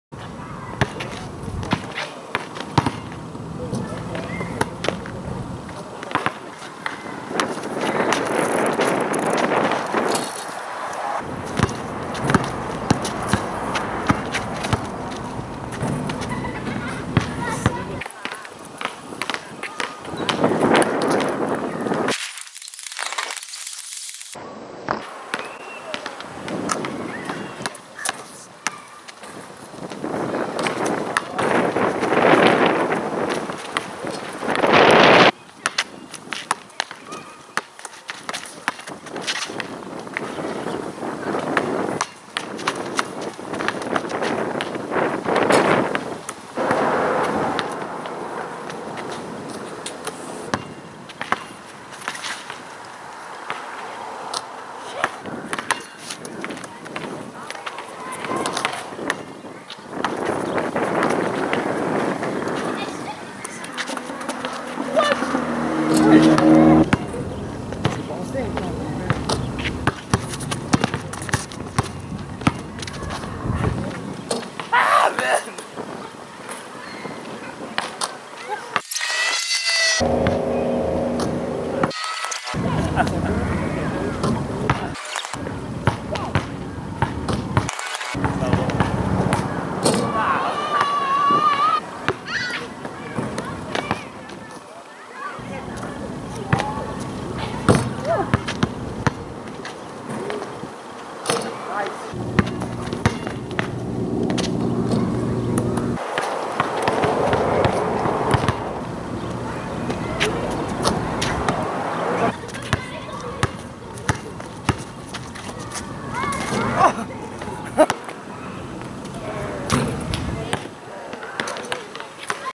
DF Playing some 1 vs 1 in Longueuil, Qc !
Just playing some 1vs 1 game with a nice guy from Montréal, Qc !